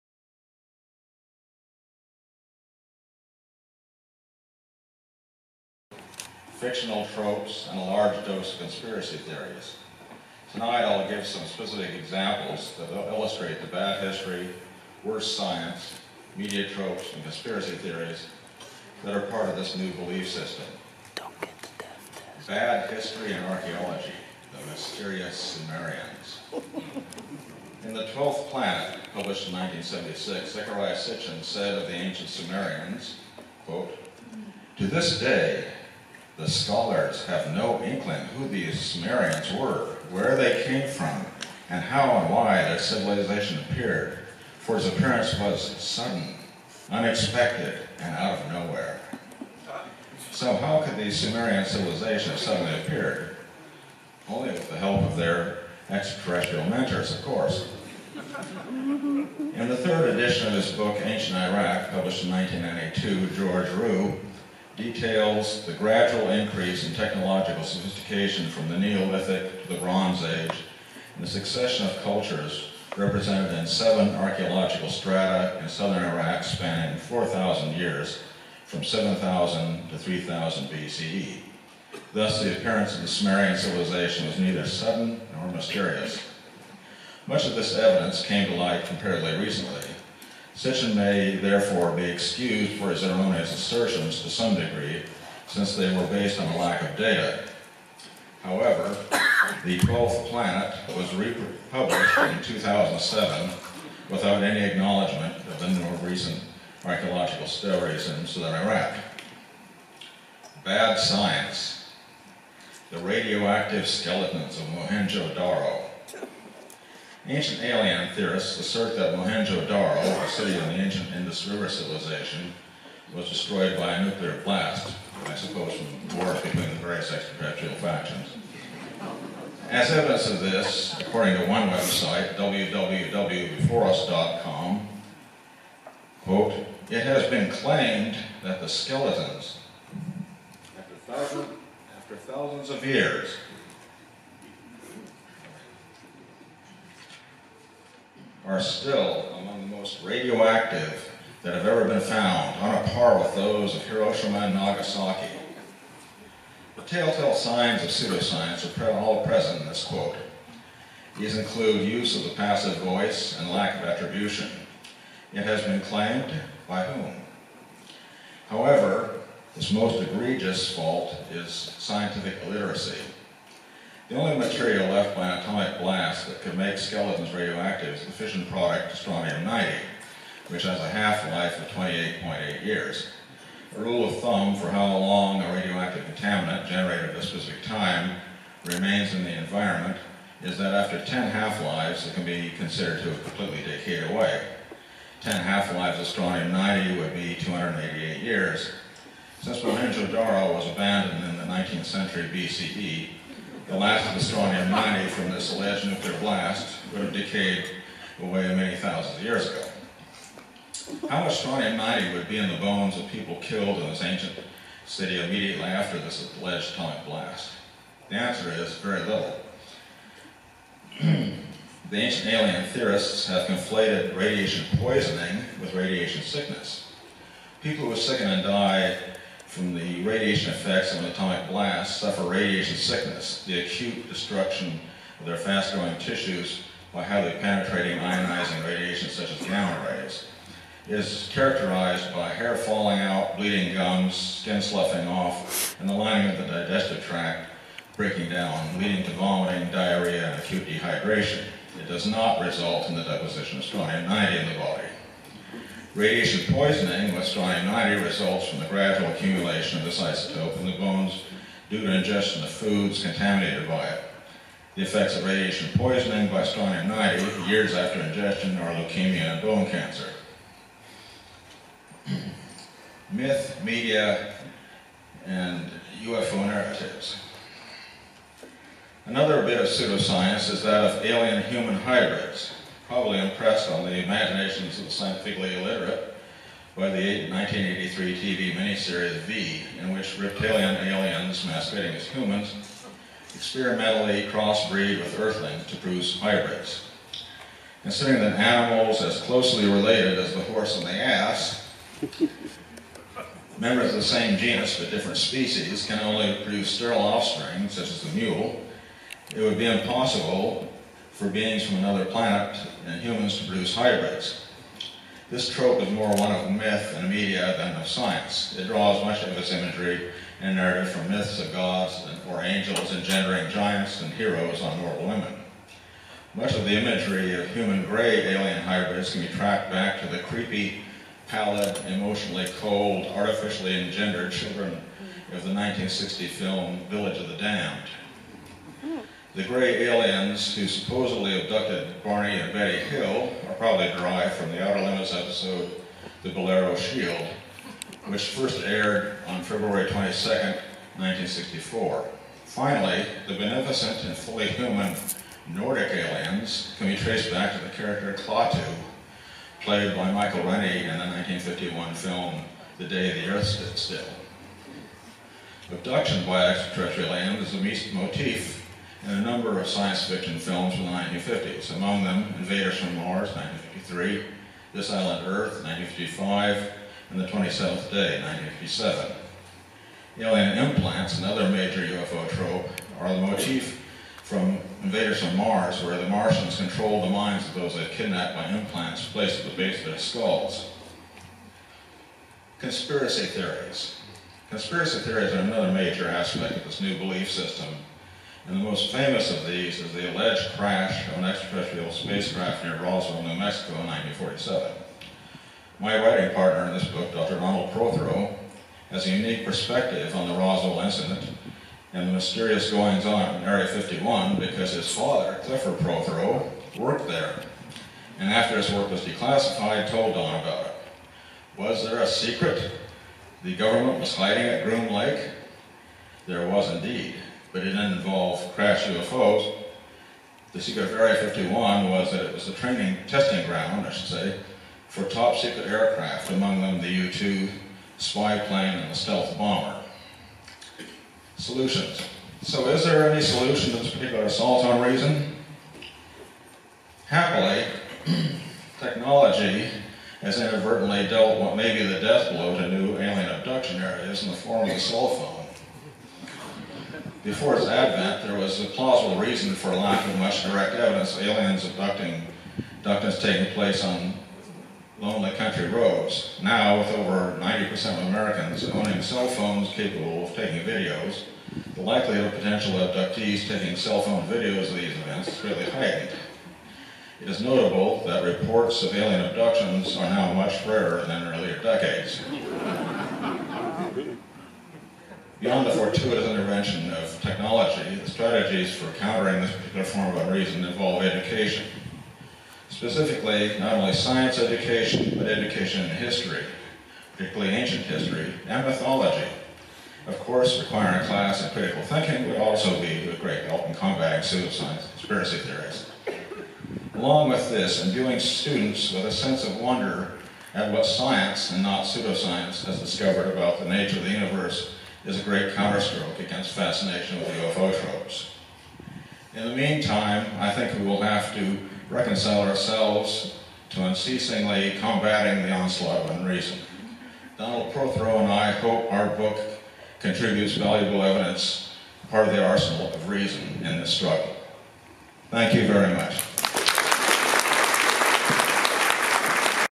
Tim Callahan receives Balles Award CSICon
Presentation of the Balles Award in Critical Thinking
Presented to Timothy D. Callahan: UFOs, Chemtrails, and Aliens